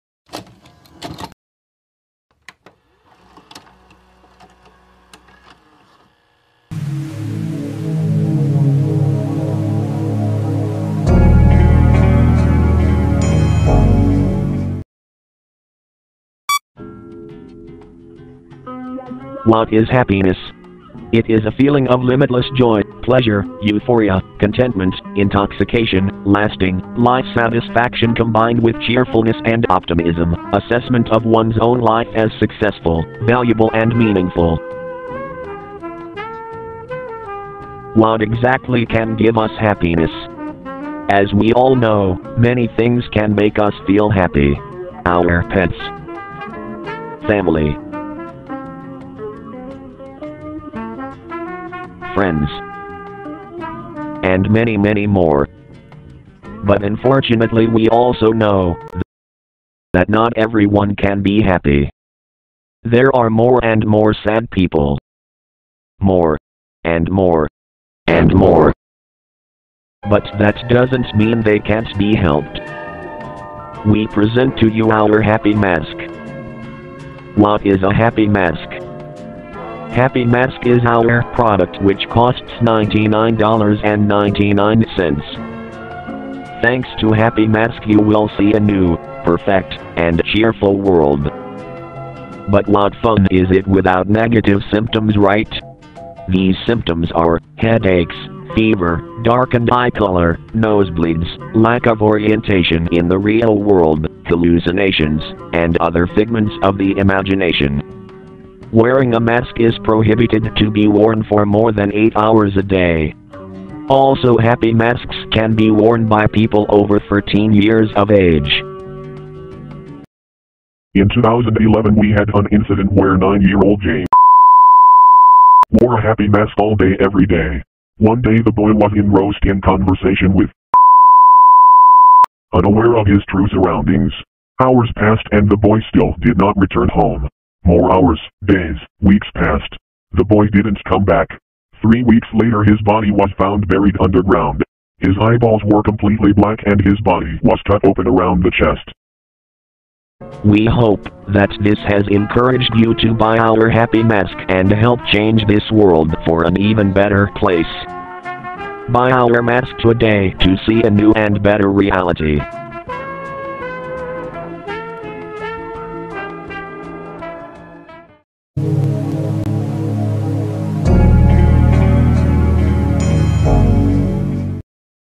Happy Mask (Short Analog Horror)
Hey there everyone i made my first analog horror video and... its cringe XD

I'm learning how to draw, edit and i Hope that my new videos of this type will be better than this.

(Also sorry for my English but I'm Polish and i don't speak it very well lol (Fun Fact: l had to sometimes use translator to translate things so sorry if there are mistakes))

Story:

Maro PL Corporation (i guess ill change it name later) is a little group of people who wants to change peoples life. It's year 2007 and they made a new thing called "Happy Mask". It's the virtual reality device which shows world without any scary things and any problems - just a Perfect, peaceful world with no danger. But it can't be this healthy... right?

Some things don't read that

Horror, analog horror, creepy, scary, mask, happy, VHS, tape